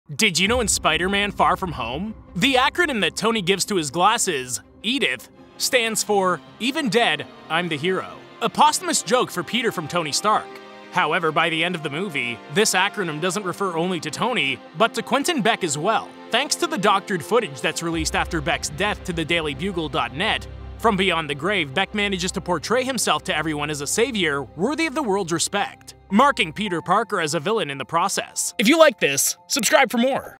Did You Know In SPIDER-MAN: FAR FROM HOME…
The acronym that Tony gives to his glasses, E.D.I.T.H, stands for “Even Dead, I’m The Hero”, a posthumous joke for Peter from Tony Stark. However by the end of the movie, this acronym doesn’t refer only to Tony, but to Quentin Beck also. Thanks to the doctored footage that is released after Beck’s death to the DailyBugle.net, from beyond the grave Beck manages to portray himself to everyone as a savior worthy of the world’s respect, marking Peter Parker as a villain in the process.